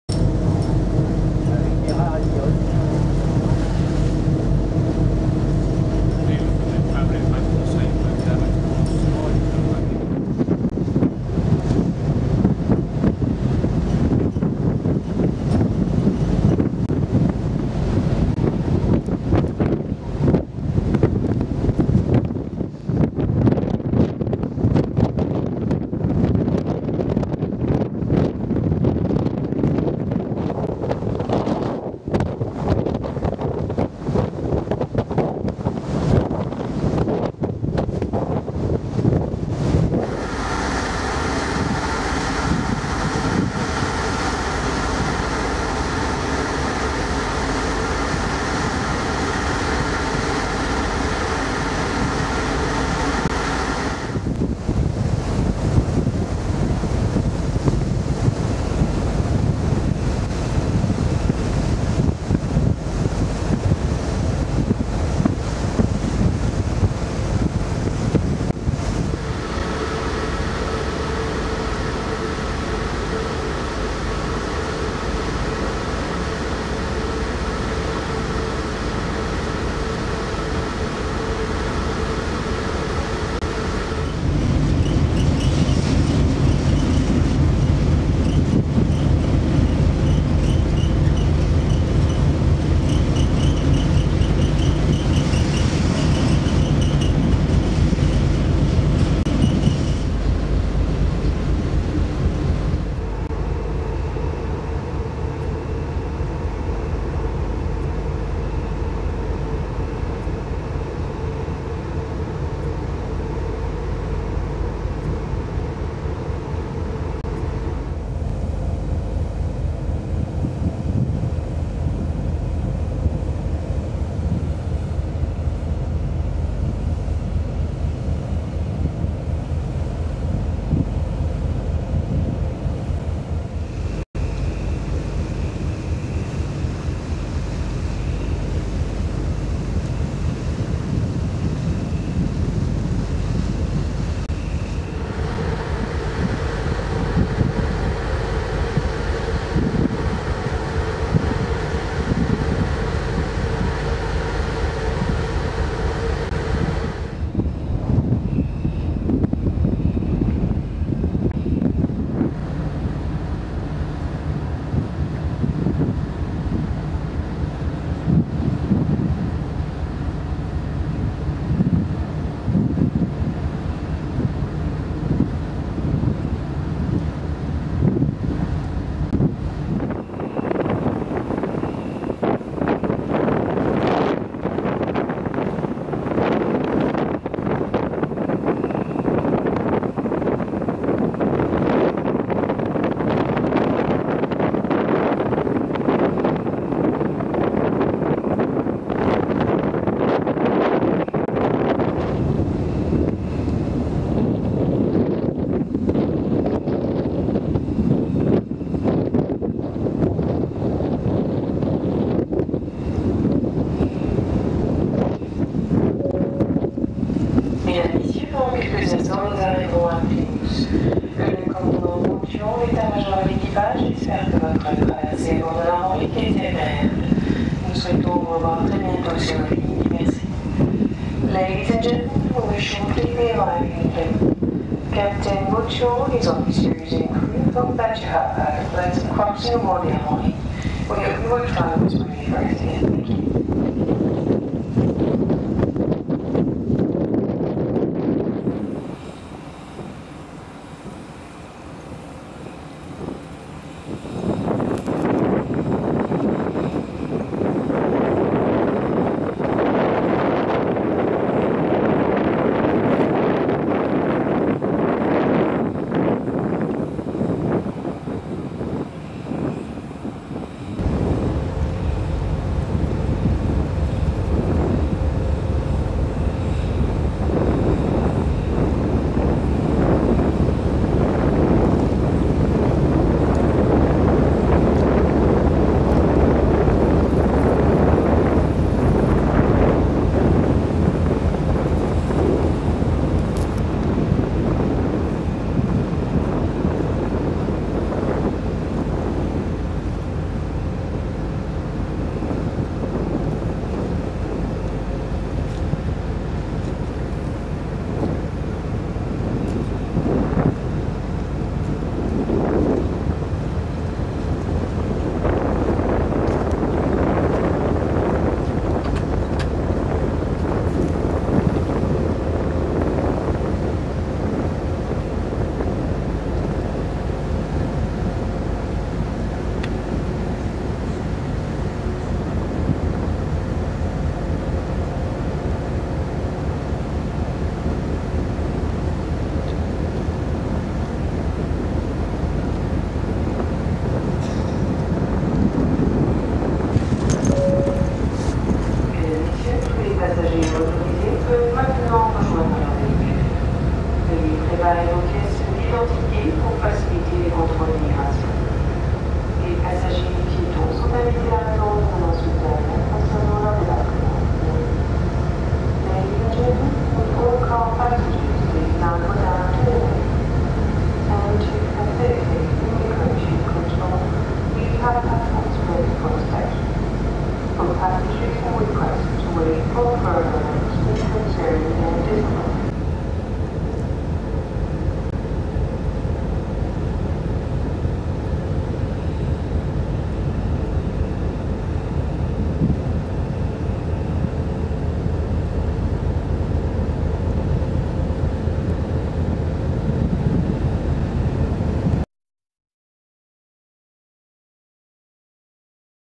Brittany Ferries MV Armorique Arriving At Plymouth, England Tuesday, 11th September 2012
Video of MV Armorique (Brittany Ferries) as it arrives at Plymouth, Devon, England from Port du Bloscon, 29680 Roscoff, Finistère, Brittany, France on Tuesday, 11th September 2012.
Vessel type: Ro-ro passenger ship
Ship Name: Armorique
Build year: 2009
IMO: 9364980
Flag: France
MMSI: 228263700
VHF Radio Callsign: FMLW